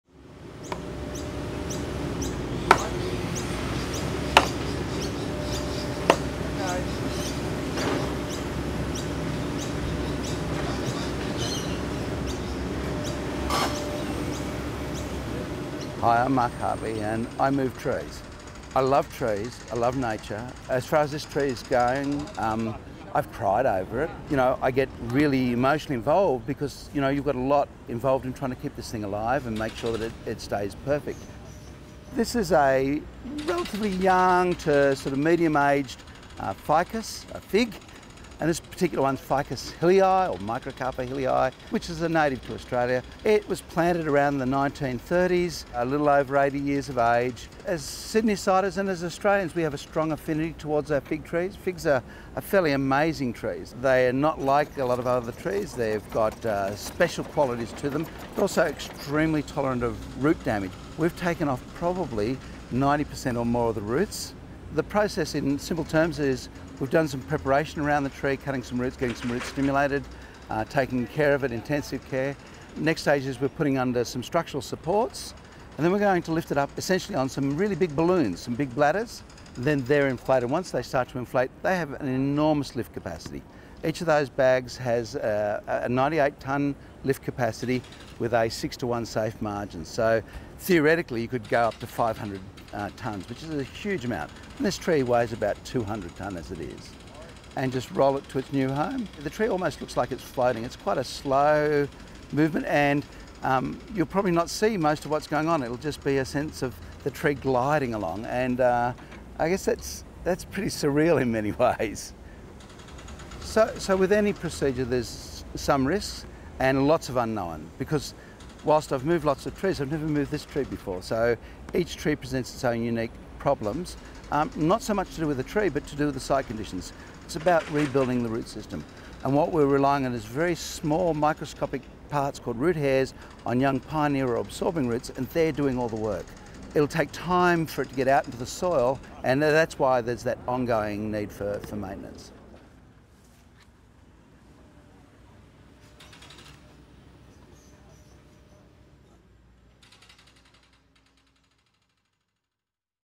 Tree Relocation for the Anzac Memorial Centenary Project
Learn how this 80-year-old fig tree has been relocated within Hyde Park.

This was completed as part of the Anzac Memorial Centenary Project.

New spaces for the community deliver enhanced education and interpretation spaces that enable the Anzac legacy.